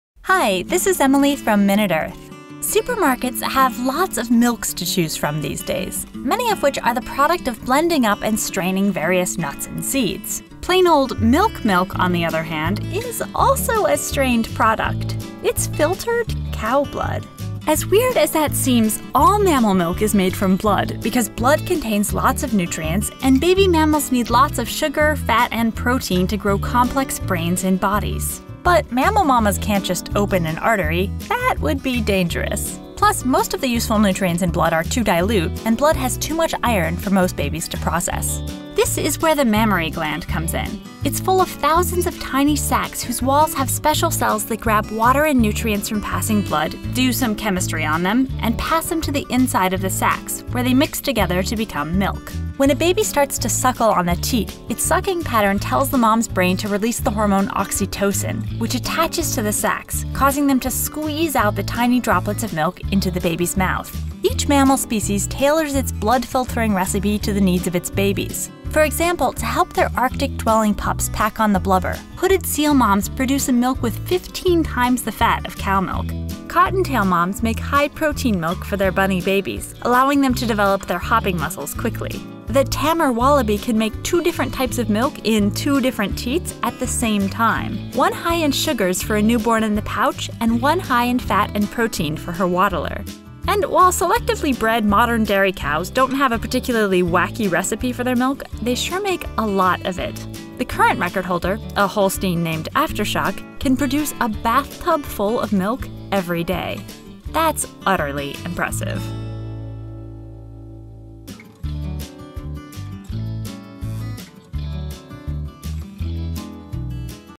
Milk Is Just Filtered Blood
Female mammals make milk, a cocktail of filtered blood, to provide their babies with vital nutrients.

To learn more about this topic, start your googling with these keywords:
- Mammal: Endothermic vertebrates that nurse their young.
- Lactation: The secretion of milk by the mammary glands.
- Mammary Gland: A cluster of alveoli and ducts that secretes milk.
- Mammary Alveoli: The tiny sacs that are the site of milk production and storage in the mammary gland.
- Oxytocin: A hormone released by the pituitary gland that - among other things - causes the milk ejection reflex during nursing.
- Holstein: A large black and white cow known for the large quantities of milk it yields.

CREDITS
Script Writer: David Goldenberg (@dgoldenberg)
Script Editor: Emily Elert (@eelert)
Video Illustrator: Qingyang Chen (@QCVisual)
Video Director: David Goldenberg (@dgoldenberg)
Video Narrator: Emily Elert (@eelert)
With Contributions From: Henry Reich, Alex Reich, Kate Yoshida, Ever Salazar, Peter Reich

REFERENCES
Skibiel, A., Downing, L., Orr, T., and Hood, W. (2013).The Evolution Of The Nutrient Composition Of Mammalian Milks. Journal of Animal Ecology. 82 (6): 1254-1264.

Tsuruta, S., Misztal, I., and Lawlor, T. (2004). Genetic Correlations Among Production, Body Size, Udder,and Productive Life Traits Over Time in Holsteins. Journal of Dairy Science. 87(5):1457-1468.

Nicholas, K. (1988). Asynchronous Dual Lactation In A Marsupial, The Tammar Wallaby. Biochemical and Biophysical Research Communications. 154 (2): 529-536.